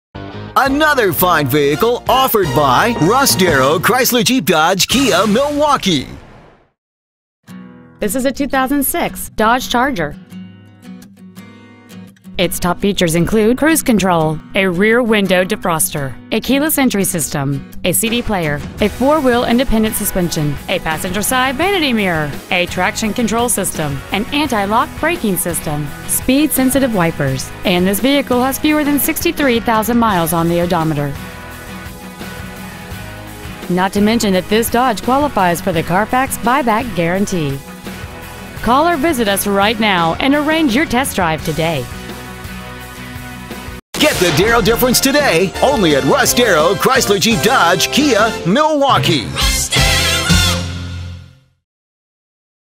2006 Dodge Charger WI
Year : 2006

 Make : Dodge

 Model : Charger

 Engine : 3.50

 Trans . : 5-Speed Automatic

 Exterior : Brilliant Black Crystal Pearlcoat

 Miles : 62,842

 Interior : Dark Slate Gray/Light Graystone

 Stock : D11053A

 Russ Darrow Chrysler Jeep Dodge Milwaukee

 7676 N. 76th St.

 2006 Dodge Charger  WI